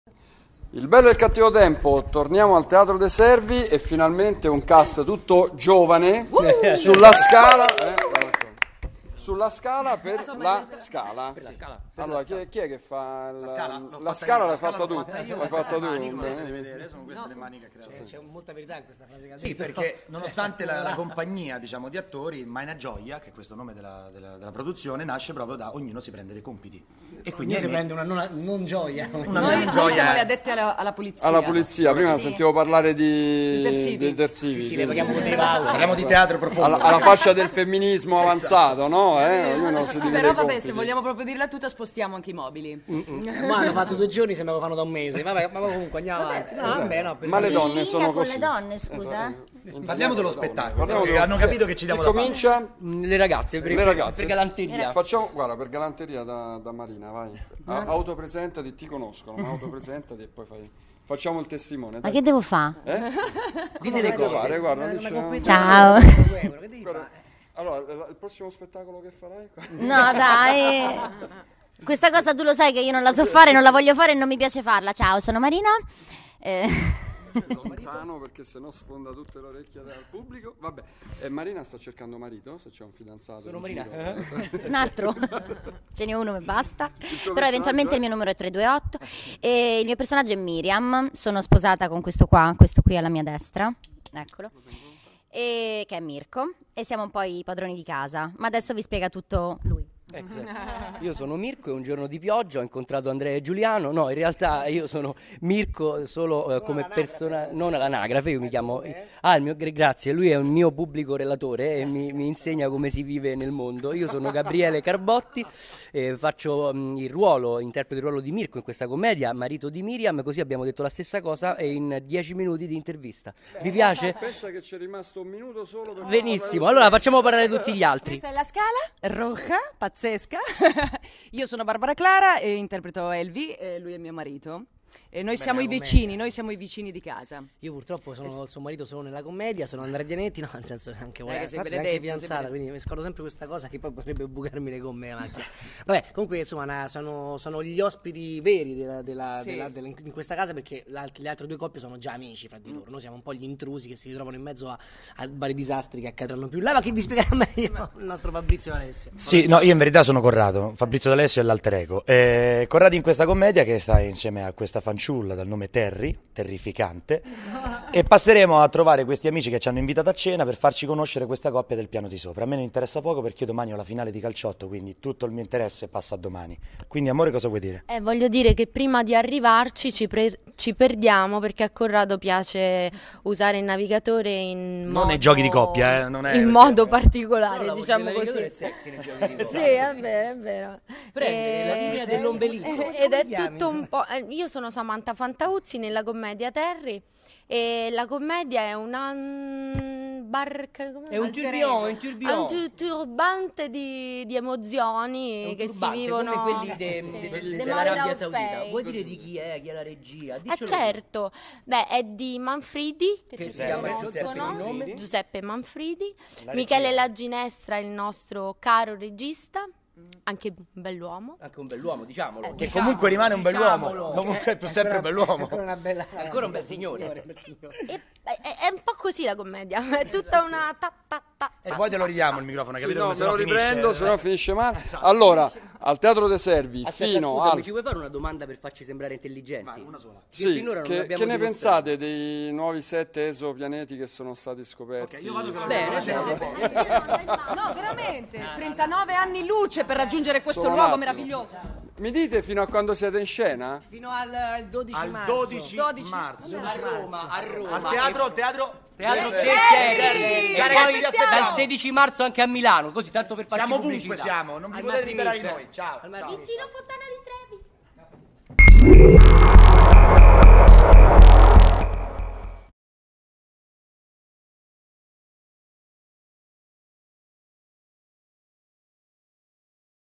La scala Teatro de' Servi
Dal 21 febbraio al 12 marzo 2017 – Teatro De’ Servi
“La scala” è un vero e proprio duello a sei, dal ritmo serratissimo, che si consuma durante un cocktail organizzato per festeggiare la ristrutturazione di un seminterrato al Nuovo Salario di cui hanno da poco preso possesso Mirko e Miriam, interpretati da Gabriele Carbotti e Marina Marchione. Con loro, una coppia di vecchi amici, Corrado e Terry (Fabrizio D’Alessio e Samantha Fantauzzi) e un’altra coppia da poco conosciuta Niccolò (interpretato da Andrea Dianetti dal cast di Amici) ed Elvi (Barbara Clara) che abita nello stesso palazzo. Fiore all’occhiello dell’approssimativo restyling, una scala che collega direttamente il seminterrato col marciapiede di fuori. Un estroso escamotage ideato per evitare, a chi debba entrare e uscire, complicati giri nel cortile esterno.
Le tre coppie si comportano come se stessero a un ricevimento della regina Elisabetta: complimenti mirabolanti, mondanità al limite dell’enfasi, smancerie esagerate, finché una piccola scintilla incendia una polveriera di emozioni represse, offese e malintesi che risvegliano vecchi scheletri tenuti nascosti nell’armadio, antichi rancori che colgono l’occasione per riemergere e colpire duro.
E sarà la scala a svolgere un ruolo determinante in questa guerra senza esclusione di colpi. La scala come simbolo, la scala come elemento architettonico, la scala come collegamento fra i piani bassi e i piani alti, insomma una vera e propria protagonista illuminata dalla pirotecnia dei dissapori umani.
Laros – MaiNaGioia Spettacoli – Matteo Fiocco presentano
“La scala”
di Giuseppe Manfridi
con Andrea Dianetti, Barbara Clara, Gabriele Carbotti, Marina Marchione Privè, Fabrizio D'Alessio, Samantha Fantauzzi
Costumi Marco Della Vecchia – Scene Augusto Sandri
Regia Michele La Ginestra
Teatro de’ Servi
Via del Mortaro, 22 Roma (ang. Via del Tritone)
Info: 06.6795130